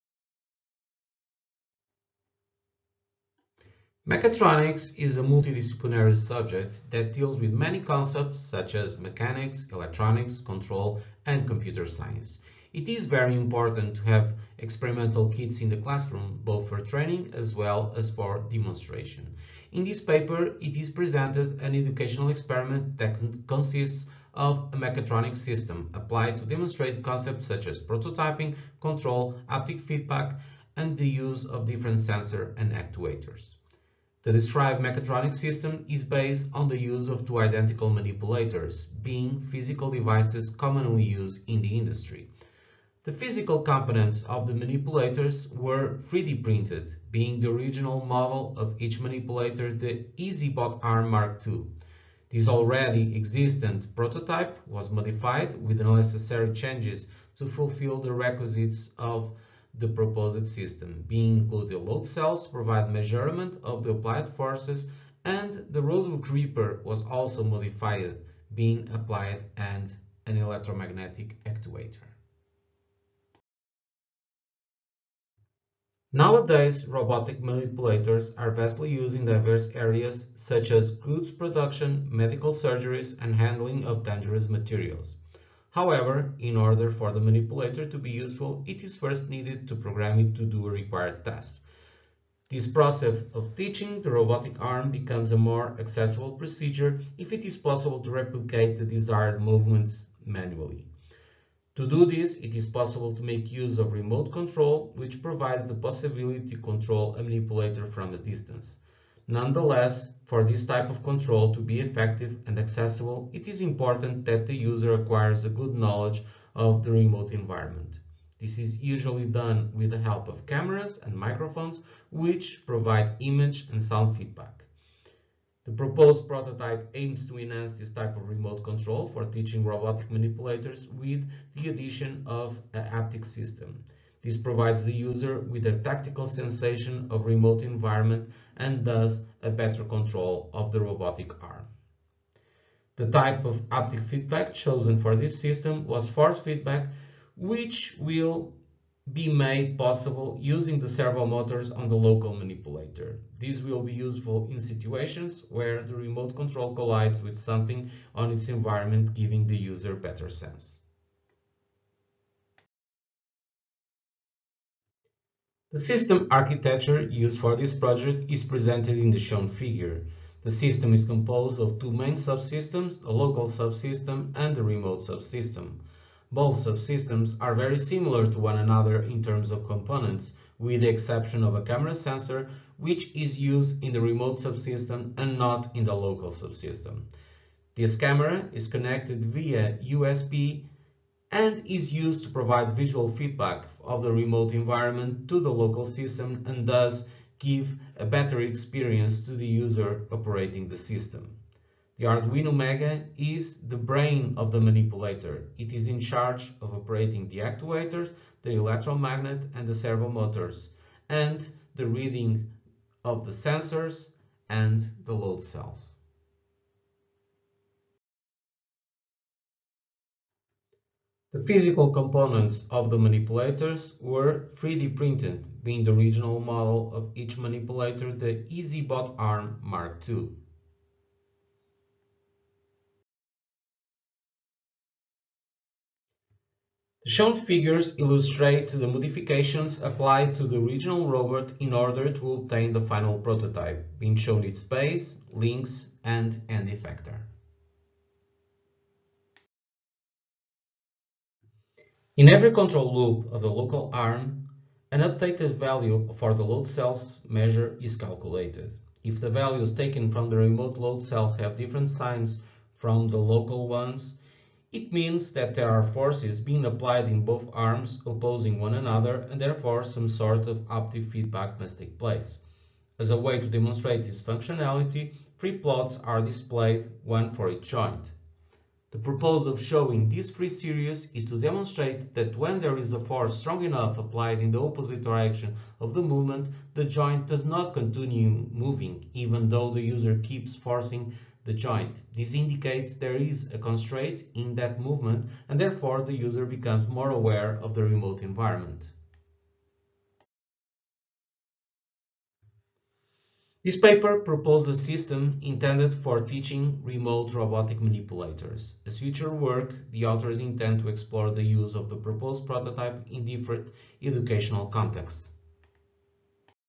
Demonstrative educational haptic manipulator robot: A teaching aid in Mechatronics
Authors: Afonso Pereira, Vítor Pinto, José Gonçalves and Paulo Costa